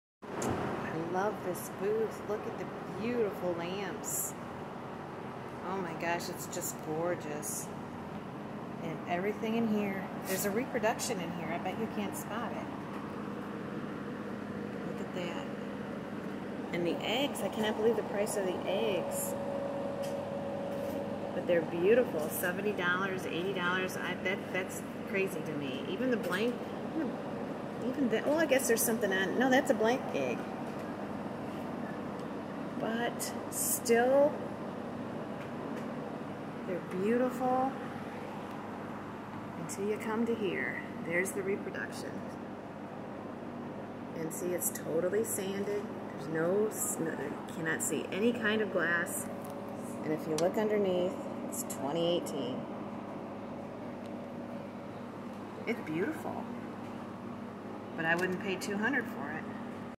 Shop with Me for Fenton Art Glass! Reproduction spotted! ~ Rosie!
clues to spotting a reproduction